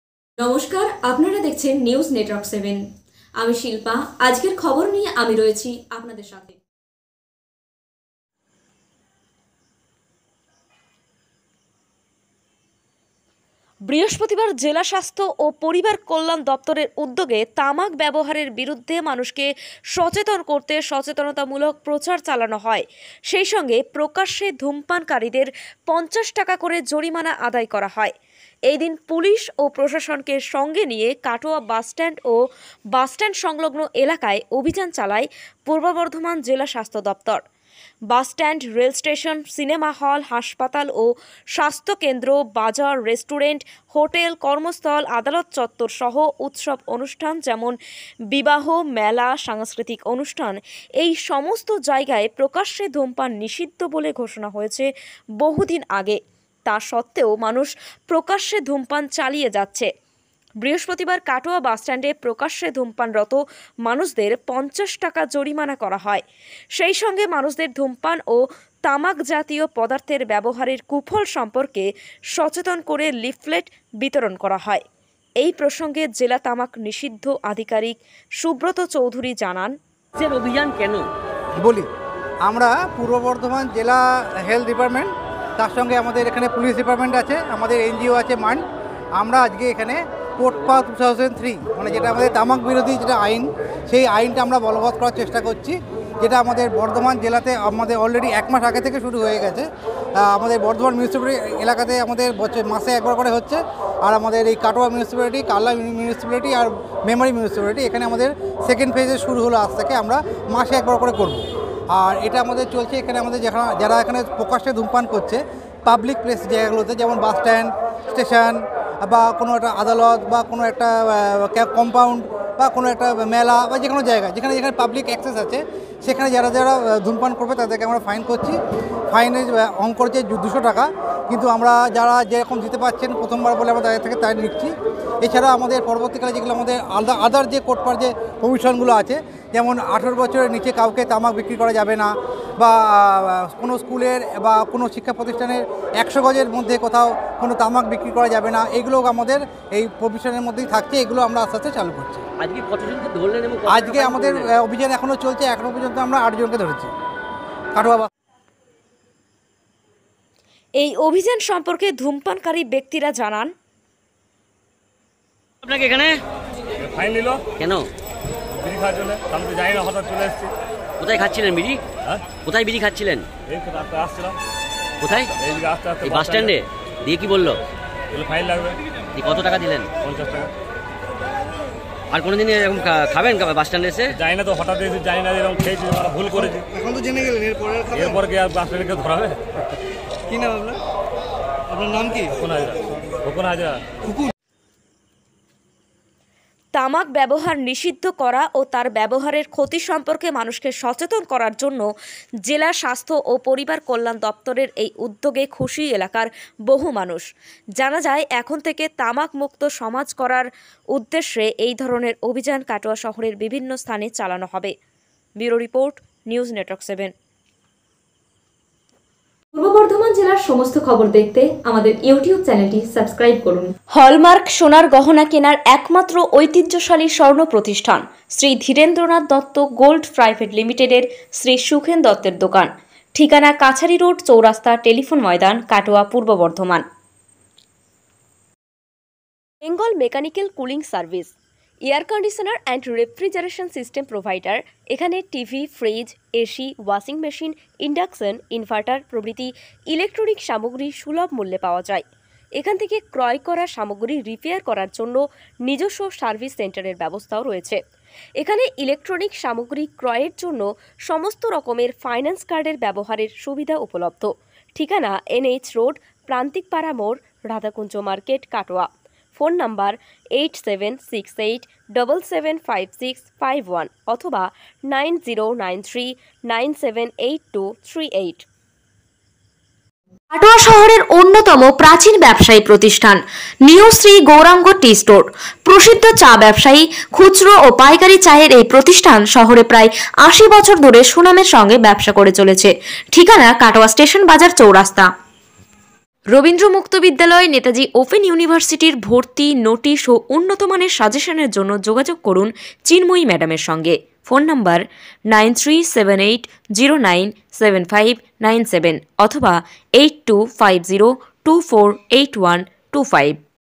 Tamak Chharun Sustho Thakun Ei Barta Niye Prochar Shuru Korlo Zilla Swasthya O Poribar Kalyan Doptor
তামাক ছাড়ুন সুস্থ থাকুন এই বার্তা নিয়ে প্রচার শুরু করলো জেলা স্বাস্থ্য ও পরিবার কল্যাণ দপ্তর 28/07/2022